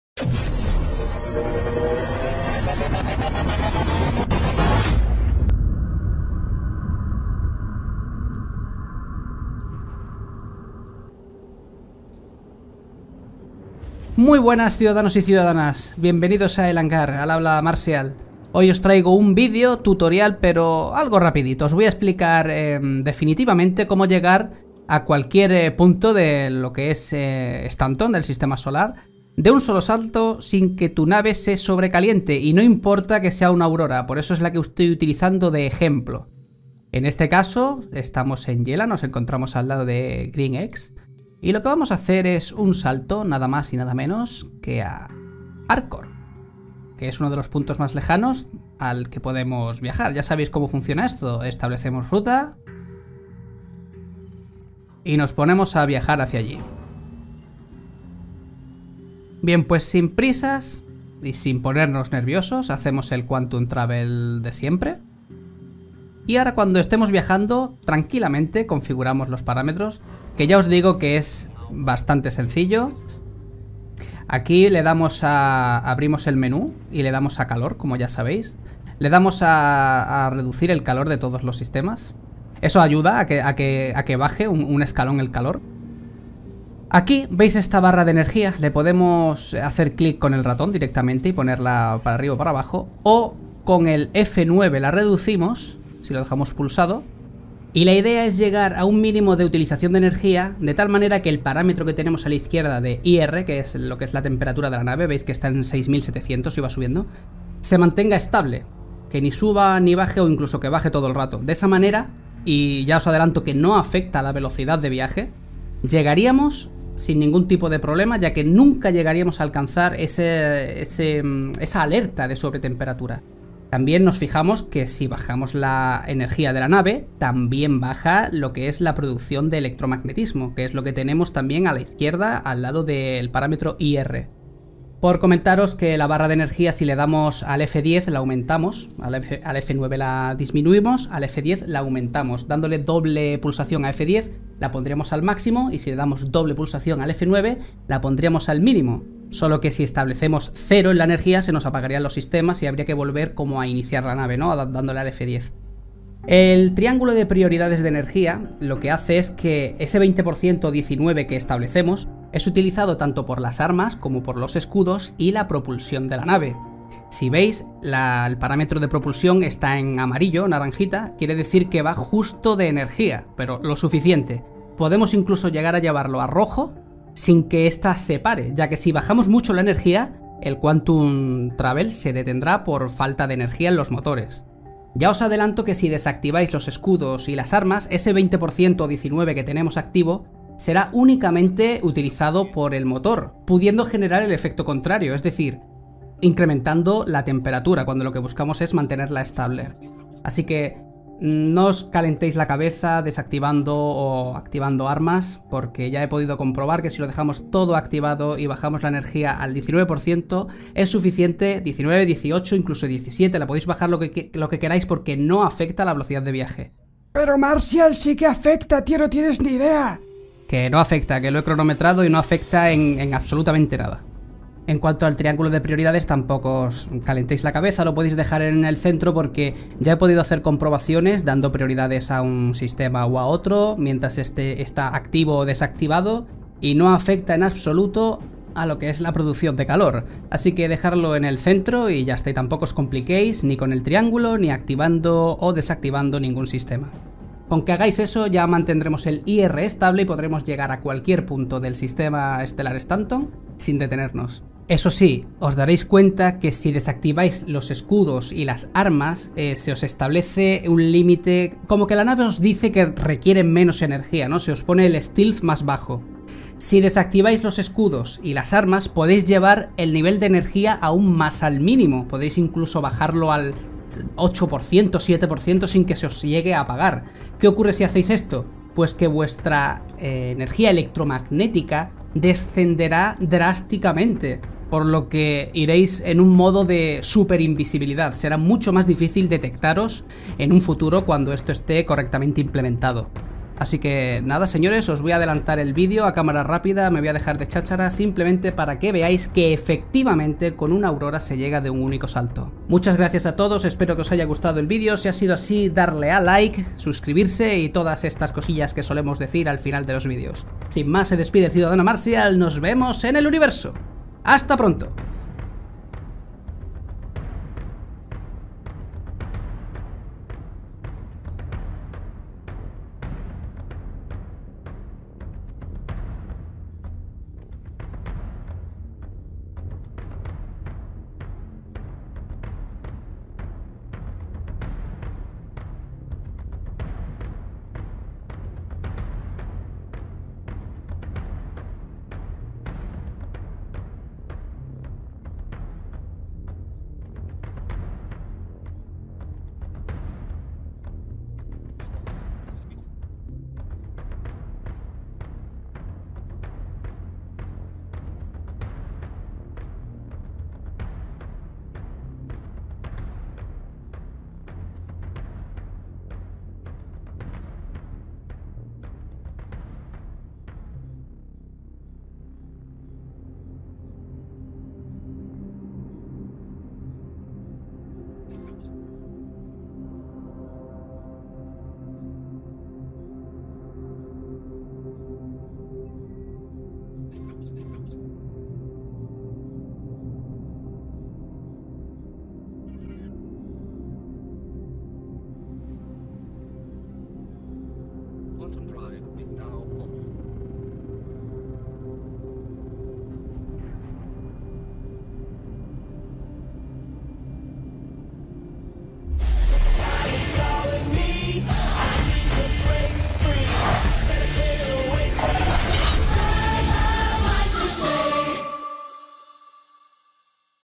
VIAJAR EN NAVE PEQUEÑA - EL HANGAR - Español
Viaja con tu nave inicial sin restricciones. Guía muy sencilla para viajar de 1 único salto!! Esperamos que os guste!!